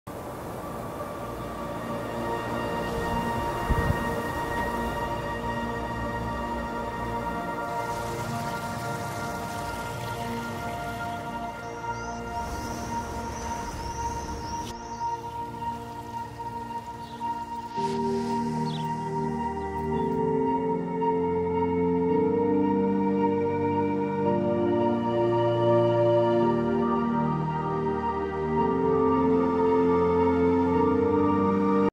Discover Villa Cariola | Lake Garda | Italy
A few and exciting seconds to discover Villa cariola, among the green hills of Lake Garda!